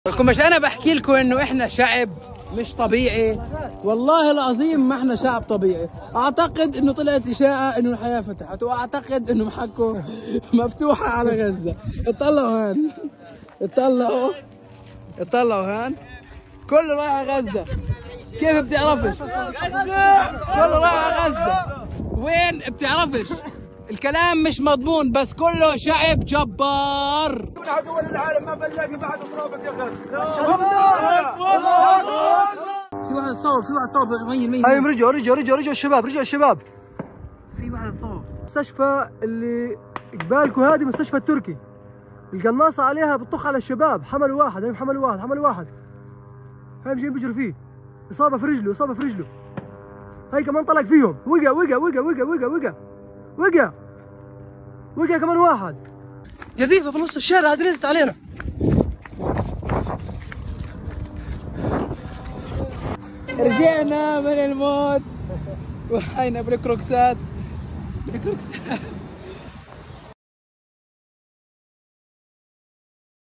Snipers Shoot At Palestinians Returning To Northern Gaza
Footage taken by Palestinians shows people returning to northern Gaza, as Israeli soldiers shoot at civilians from a hospital nearby.